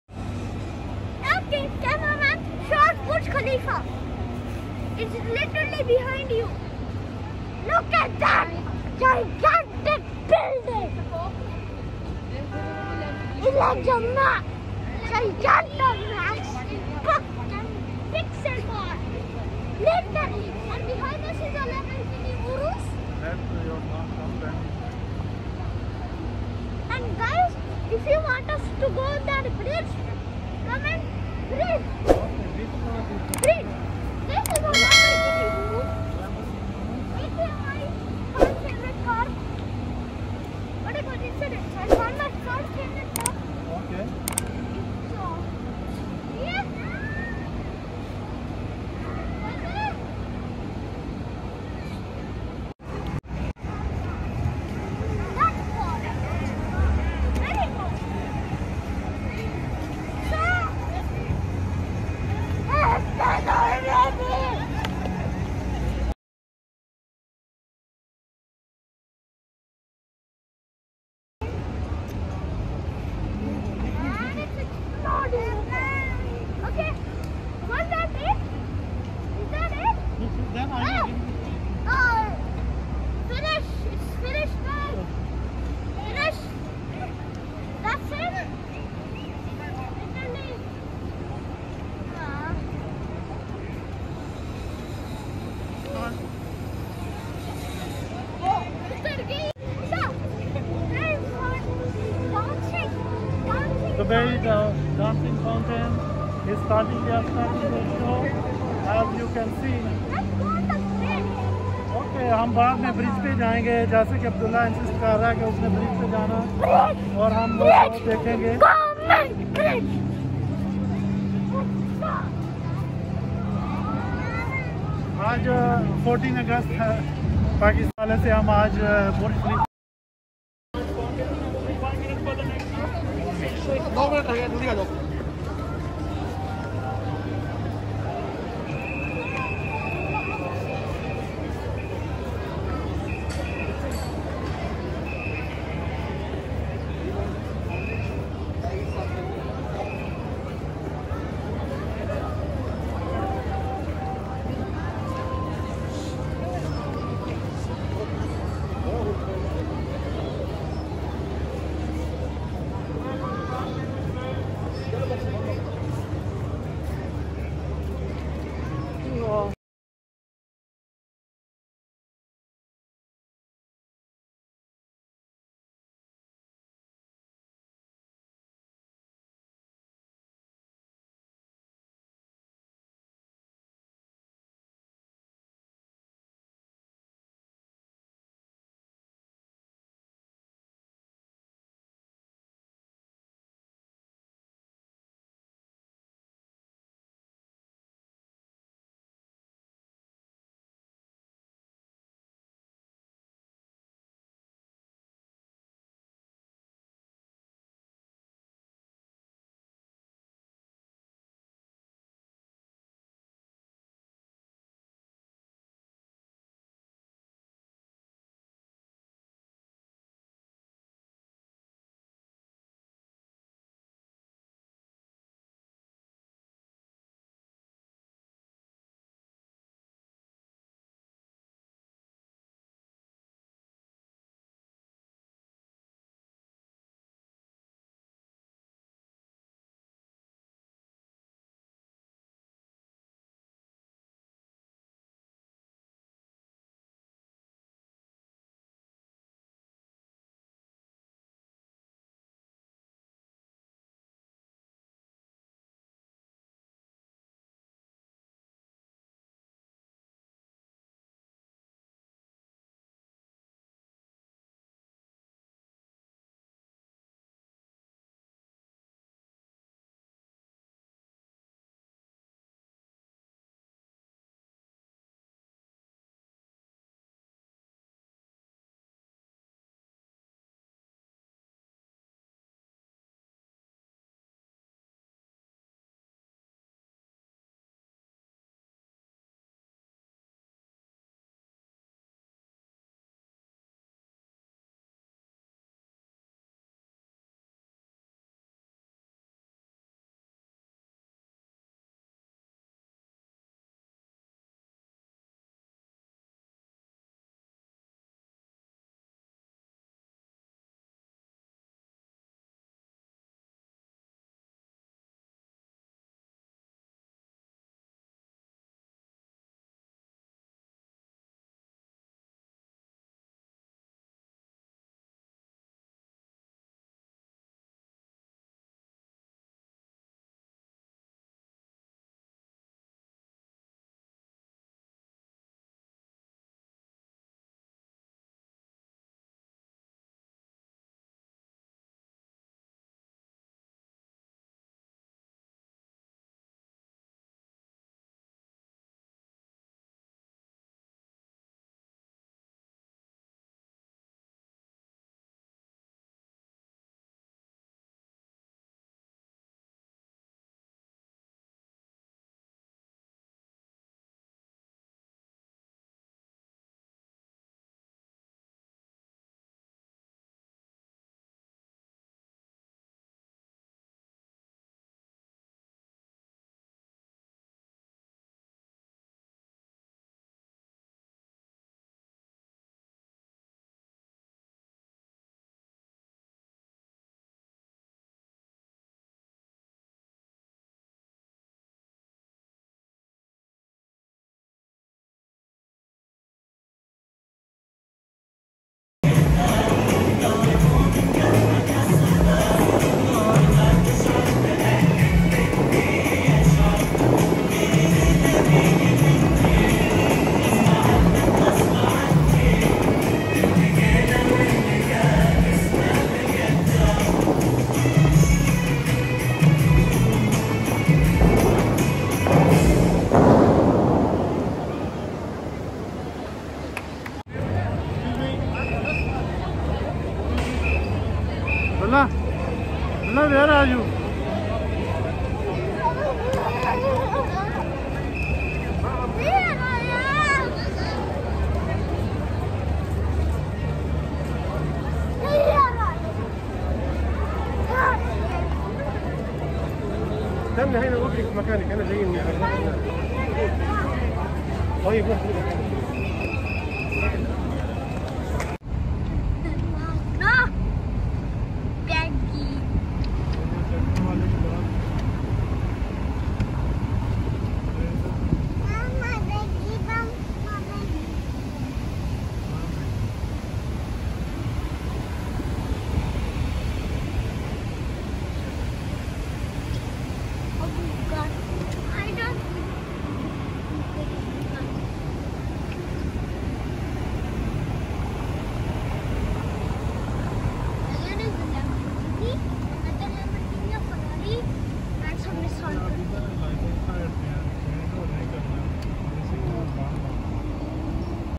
Amazing Tour to The Burj Khalifa Fountain Dance With Abdullah
The Dubai Fountain is the world's largest choreographed fountain system set on the 30-acre man made Burj Khalifa Lake, at the centre of the Downtown Dubai development in Dubai, United Arab Emirates. This spectacular fountain package is 246 ft long and shoots water up to 500 ft into the air accompanied by a range of classical to contemporary Arabic and world music.
These are even better than the ones in Las Vegas. Dubai is so beautiful